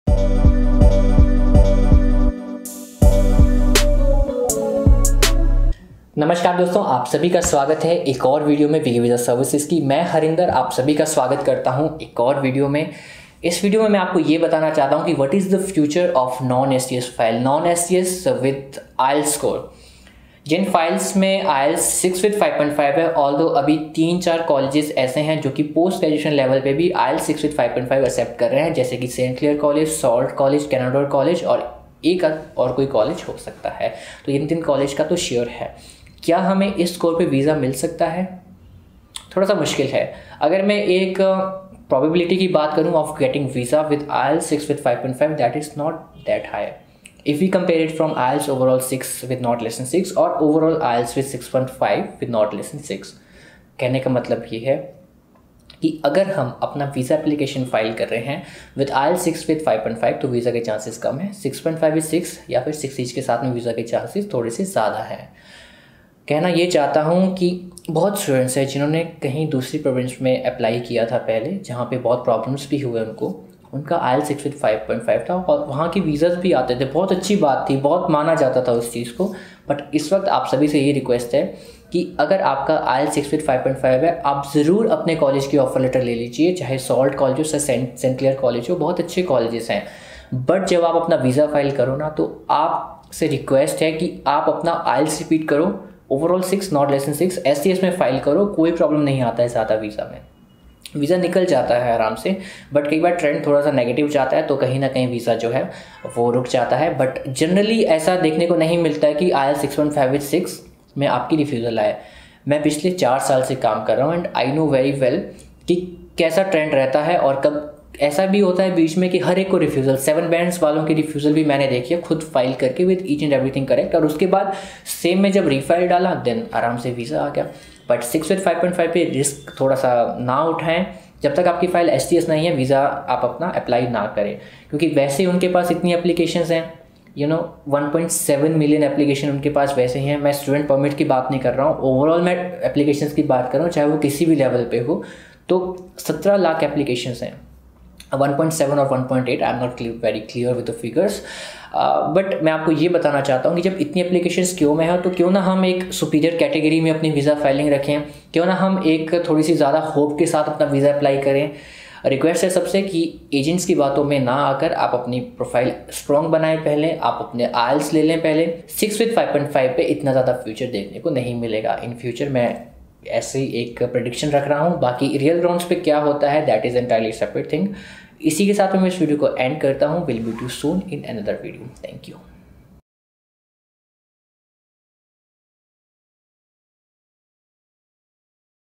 FUTURE of NON-SDS Files of CANADA with 6 (5.5) | without GIC and 6 Months Fee !!!
Address- Inside Piccadily Square, 108 First Floor, Sector 34A, Chandigarh.

Google Maps Link- shorturl.at/qBRTV
Landline Number- (11 AM to 6 PM) - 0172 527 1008